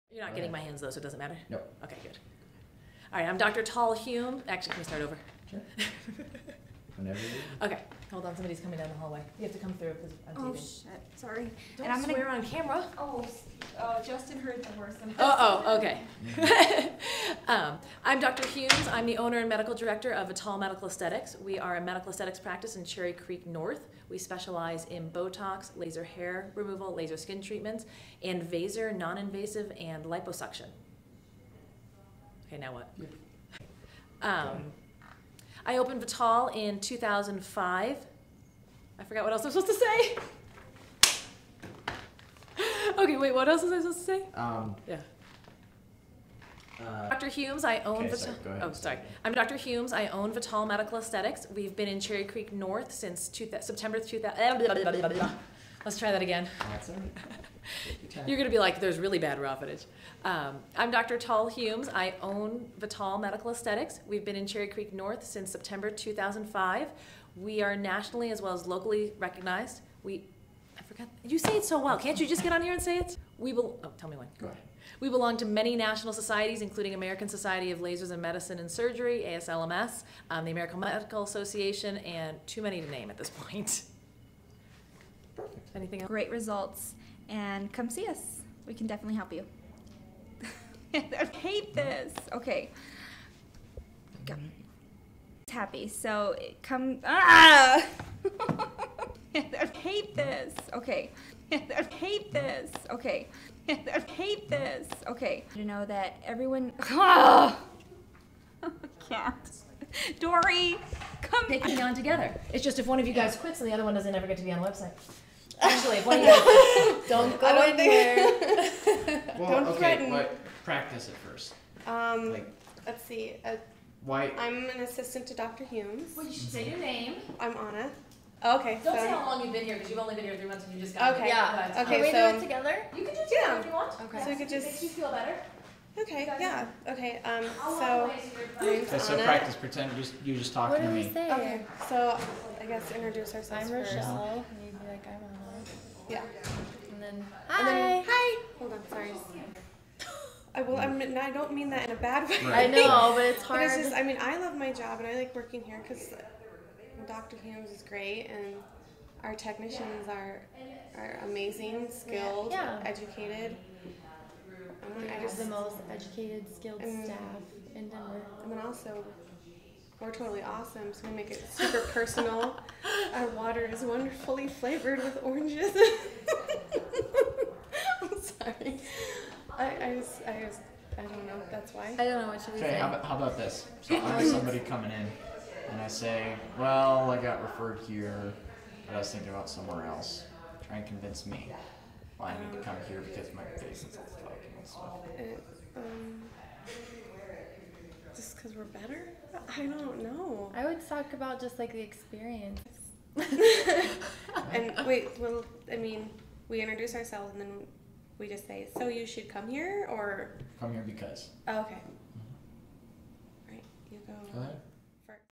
VITAHL Staff Bloopers
VITAHL staff  show they aren't as smooth on camera as they may seem. Skin is our business.